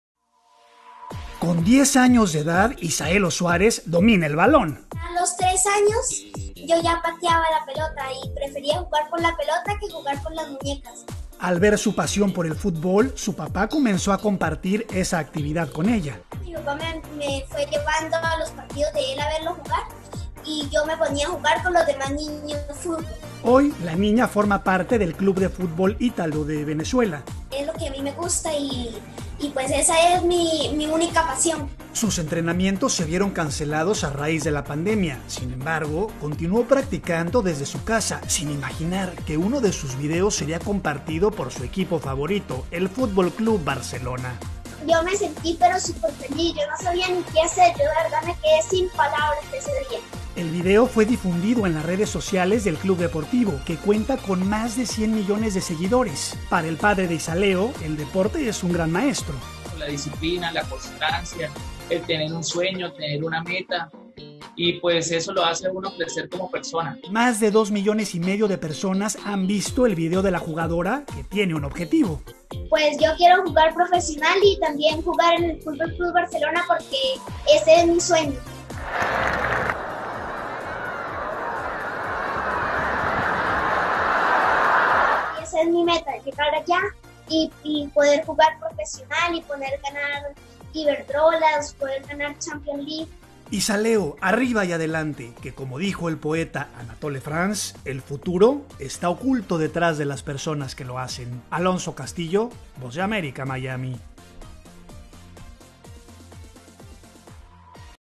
Isabella Suárez, niña que domina el balón de fútbol
¡Visítenos!